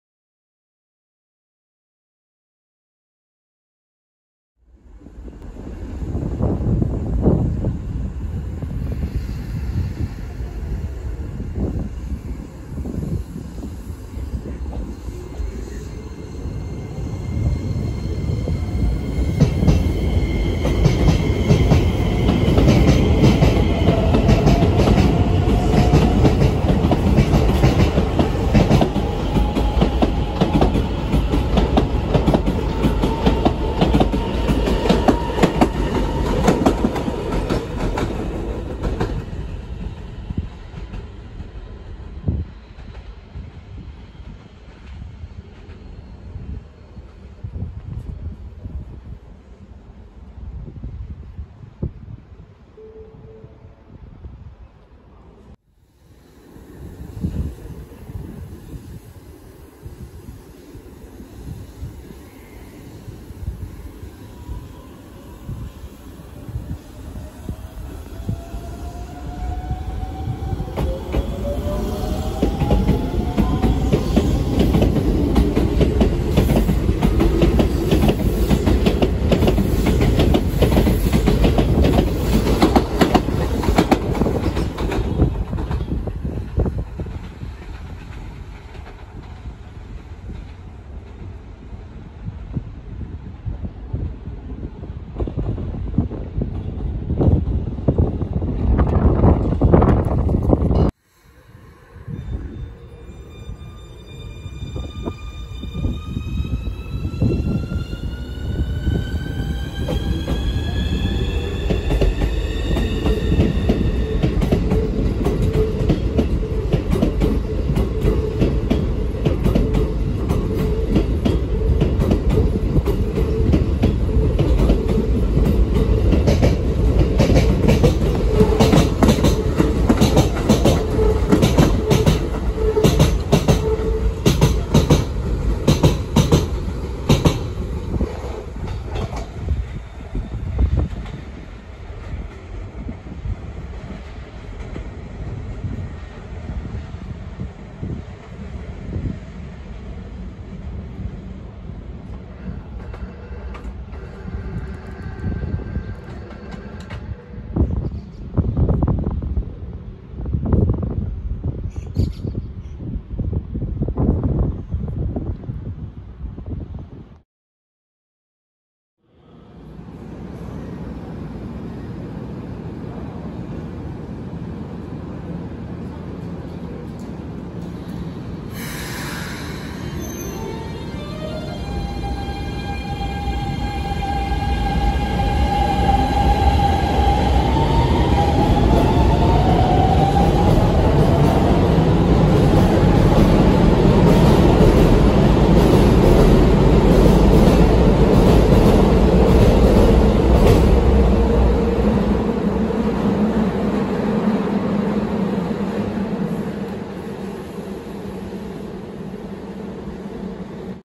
［青砥駅］レアな車両ばかり⁉️鳥鉄が追いかける？青砥駅撮影第2弾
2回目の青砥駅での撮影です。レアな車両ばかりなので、お楽しみいただけますと幸いです
鳥鉄とは3:16の事です